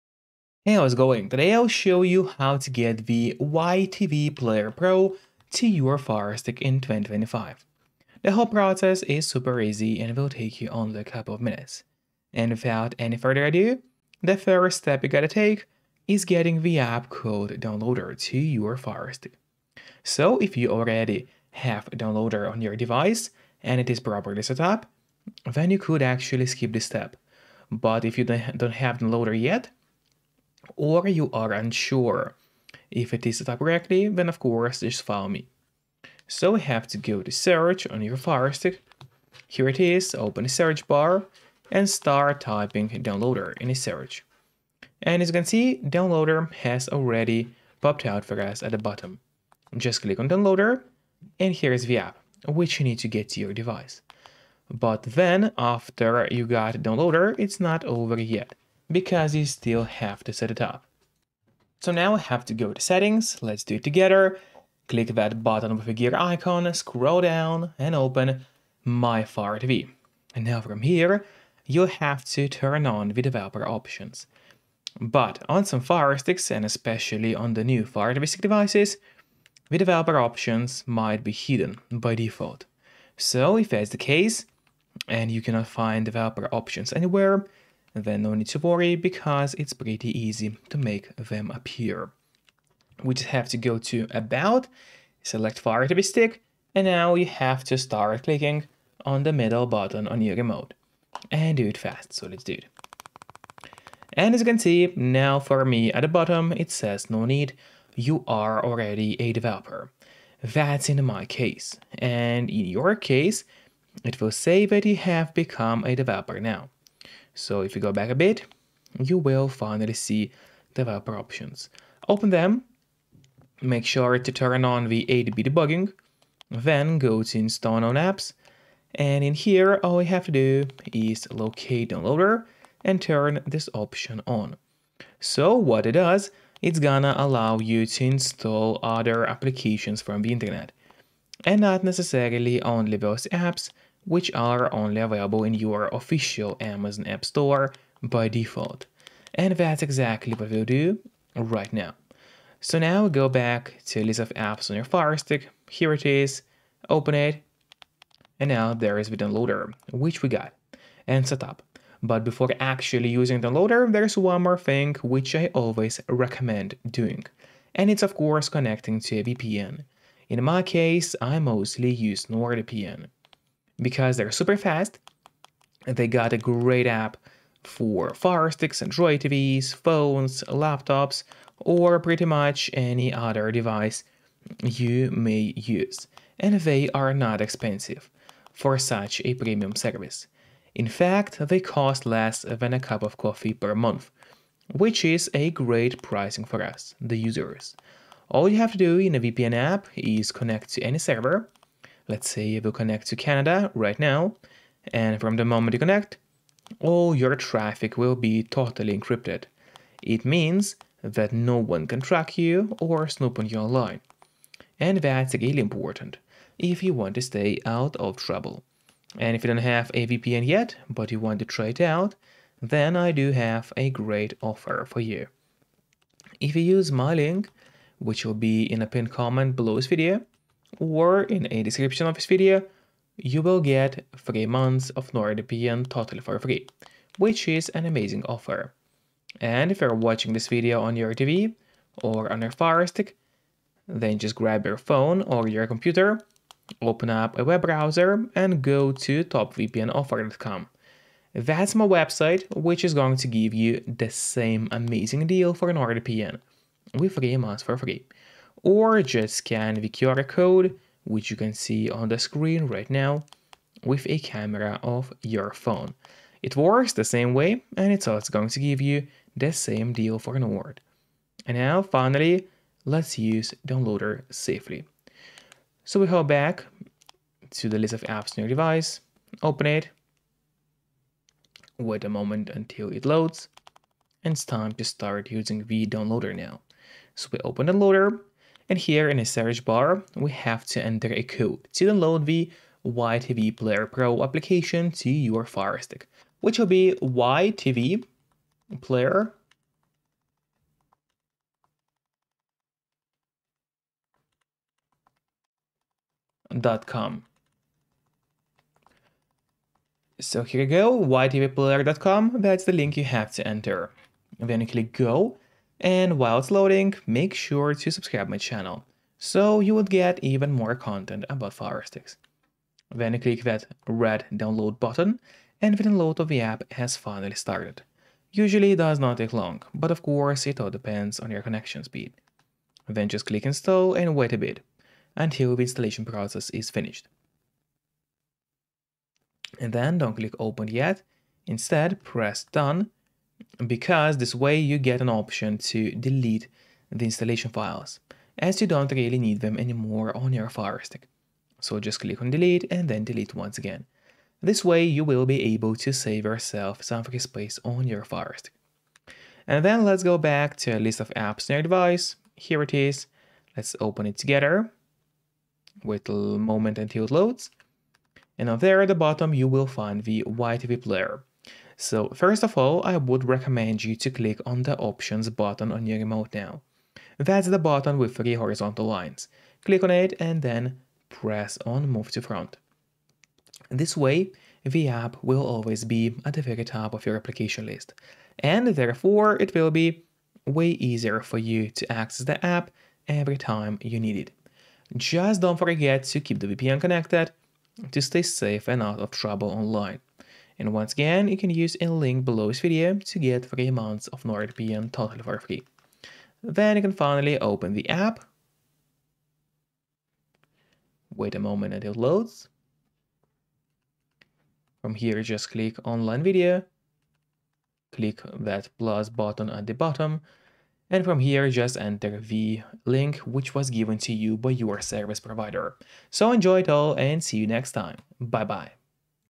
How to Download YTV Player Pro to Amazon Firestick (in 2025)
Follow these steps to download and install YTV Player pro app to a Firestick. That's the easy way to download YTV Player on any Amazon Firestick.